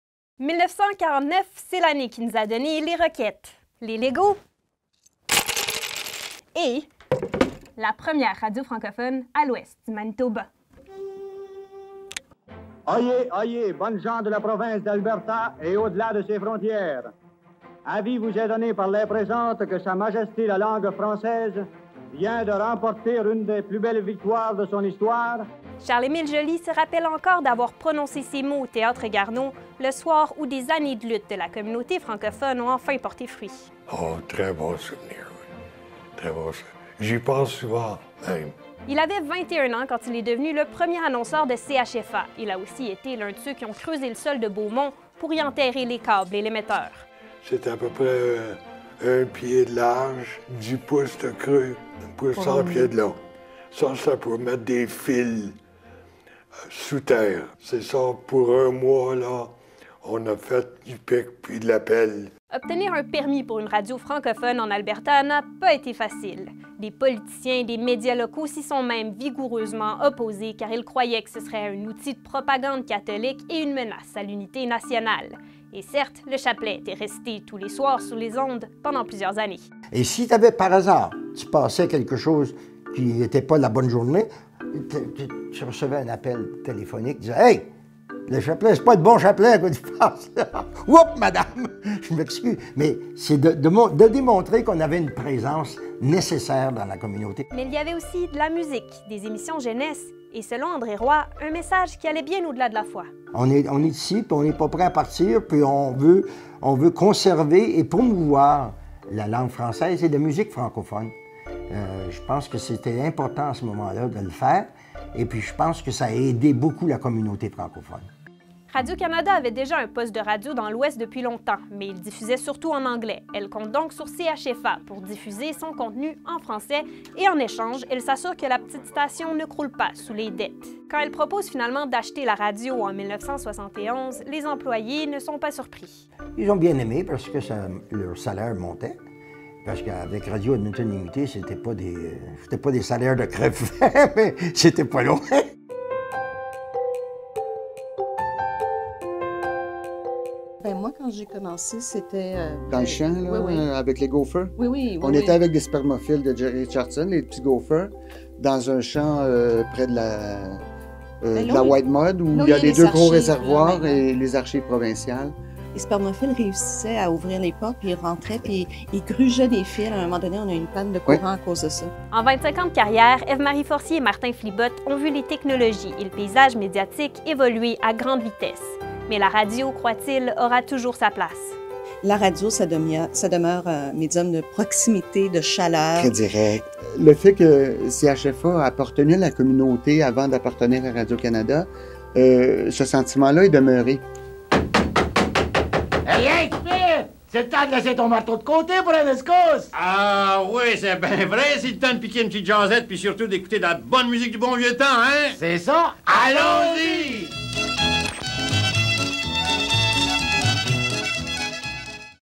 CHFA À 70 ANS
En 1949, alors que le monde découvrait les blocs Lego et les disques 45 tours, les Albertains obtenaient leur première chaîne de radio francophone. Ainsi a débuté l'histoire d'une petite radio communautaire appelée CHFA, qui est aujourd'hui celle d'ICI Alberta.